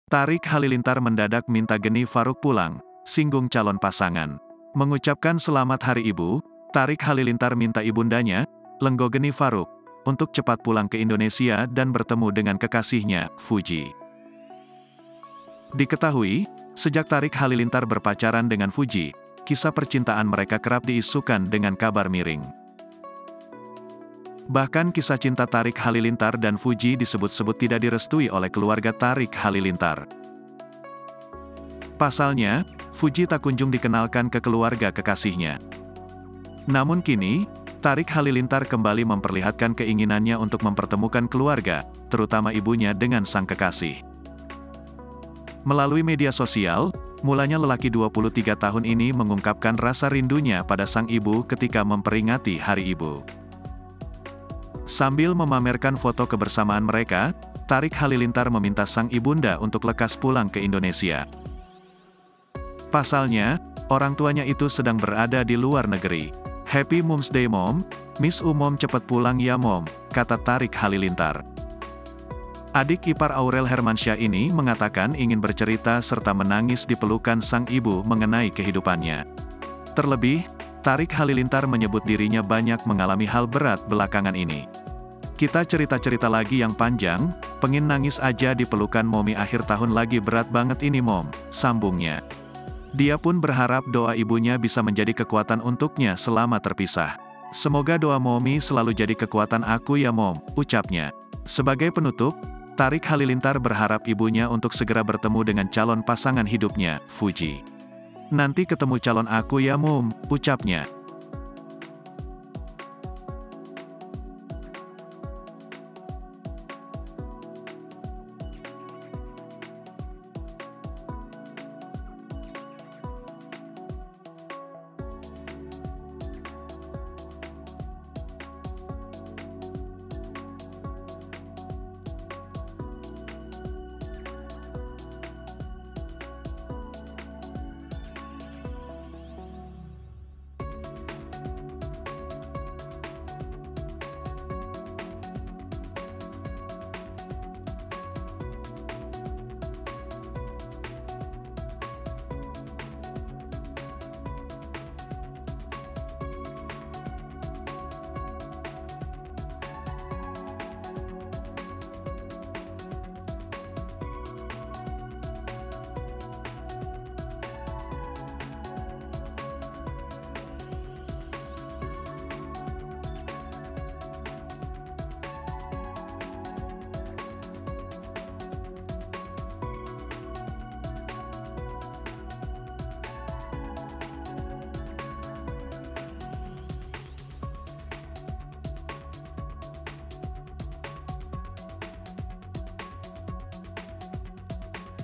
Thariq Halilintar Mendadak Minta Geni Faruk Pulang, Singgung Calon Pasangan
- Mengucapkan Selamat Hari Ibu, Thariq Halilintar minta ibundanya, Lenggogeni Faruk, untuk cepat pulang ke Indonesia dan bertemu dengan kekasihnya, Fuji.
Diketahui, sejak Thariq Halilintar berpacaran dengan Fuji, kisah percintaan mereka kerap diisukan dengan kabar miring.
Bahkan kisah cinta Thariq Halilintar dan Fuji disebut-sebut tidak direstui oleh keluarga Thariq Halilintar.
Pasalnya, Fuji tak kunjung dikenalkan ke keluarga kekasihnya.
Namun kini, Thariq Halilintar kembali memperlihatkan keinginannya untuk mempertemukan keluarga, terutama ibunya dengan sang kekasih.
Sambil memamerkan foto kebersamaan mereka, Thariq Halilintar meminta sang ibunda untuk lekas pulang ke Indonesia.
Pasalnya, orangtuanya itu sedang berada di luar negeri.
"Happy mums day mom, miss u mom cepet pulang yaa mom," kata Thariq Halilintar.